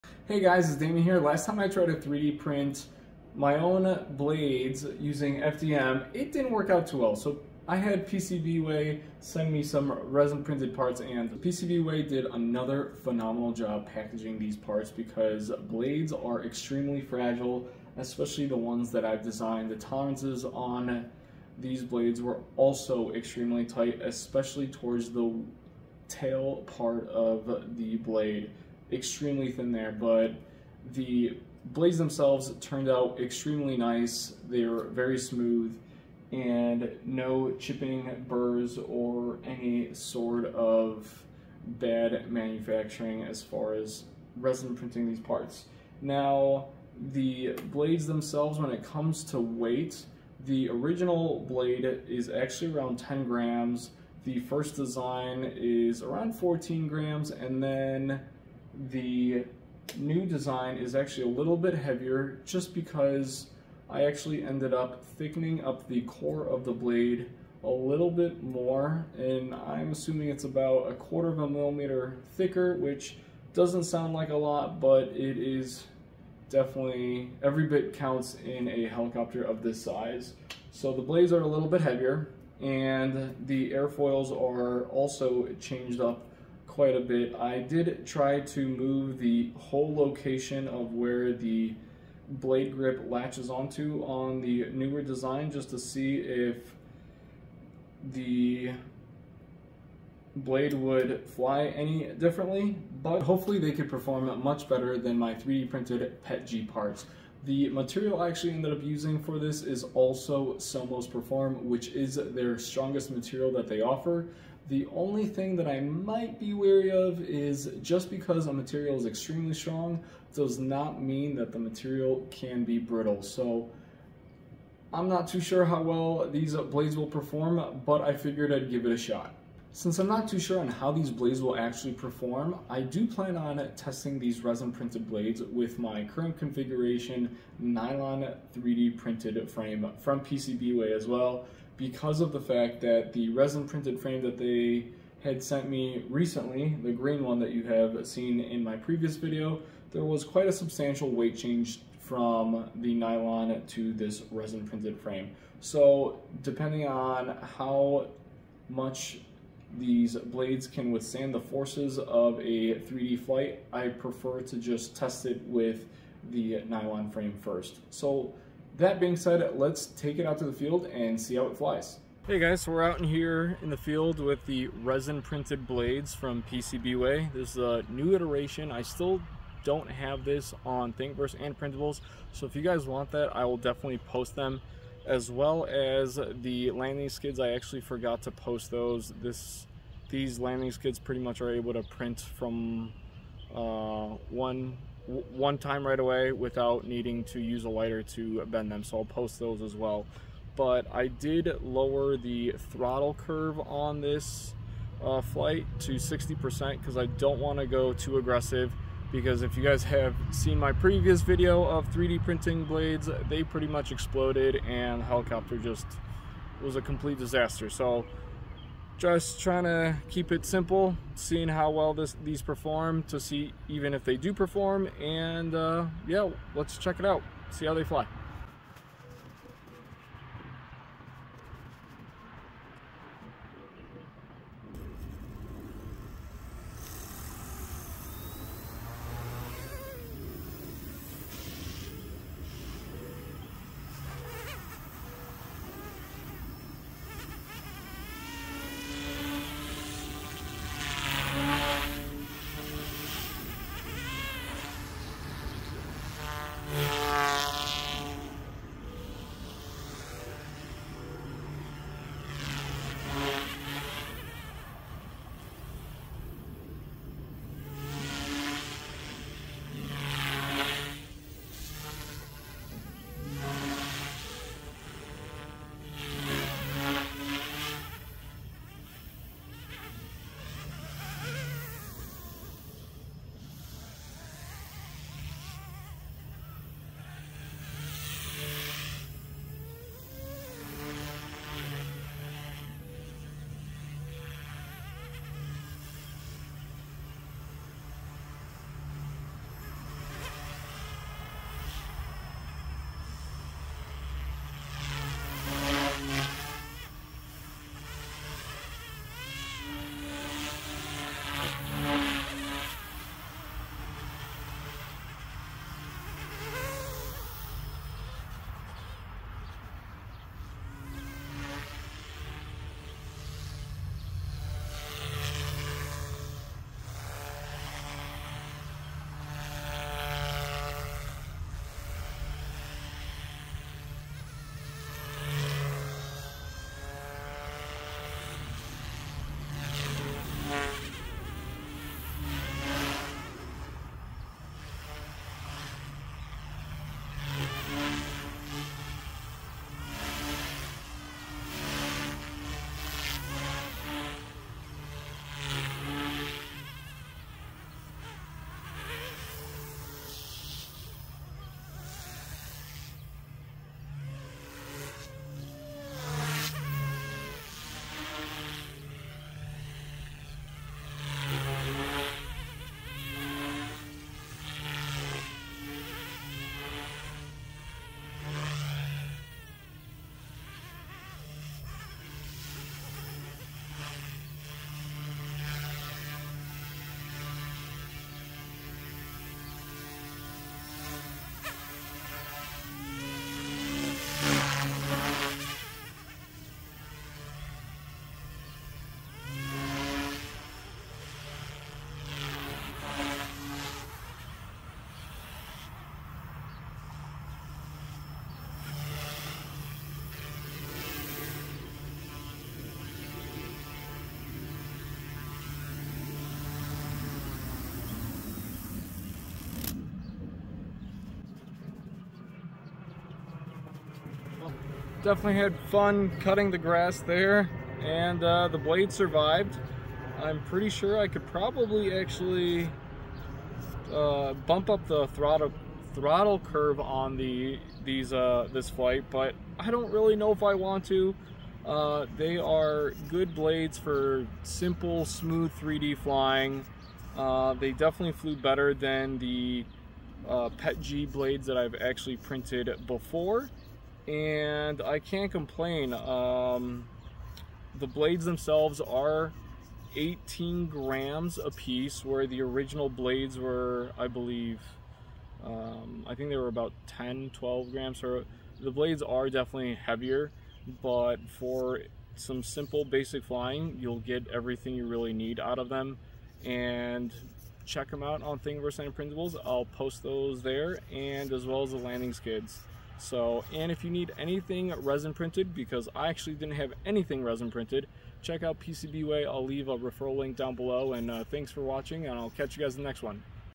Can 3D Printed Helicopter Blades Take Flight???
My mystery parts were blades! Let's see how they look and fly!